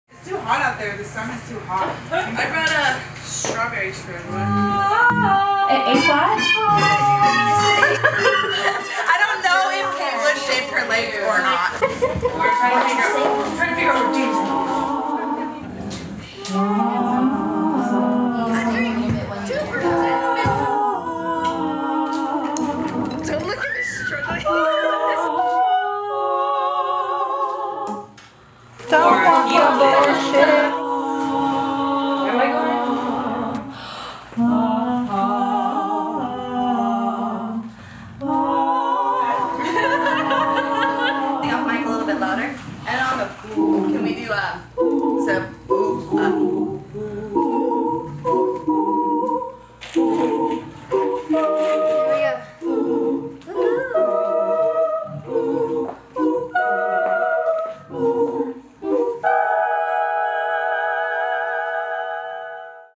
strawberries for everyone
The giggles, smiles, soundcheck and hilarious commentary of eight bushes getting ready to rehearse the music of Kate Bush in the spring of 2019.